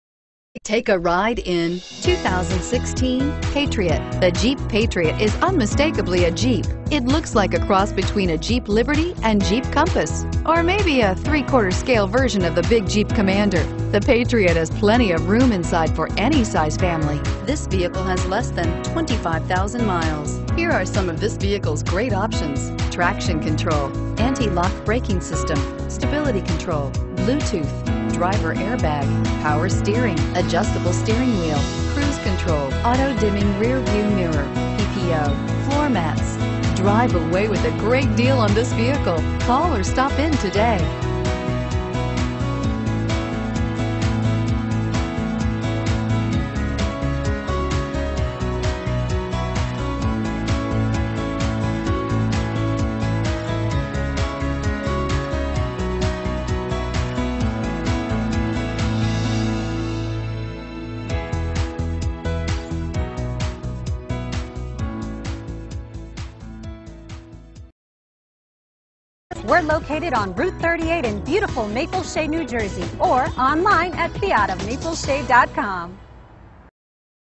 2016 Jeep Patriot Cherry Hill NJ 77530
2016 Jeep Patriot Sport SE

Looking for the right sport utility? Today could be your lucky day. this 2016 Jeep Patriot could be yours.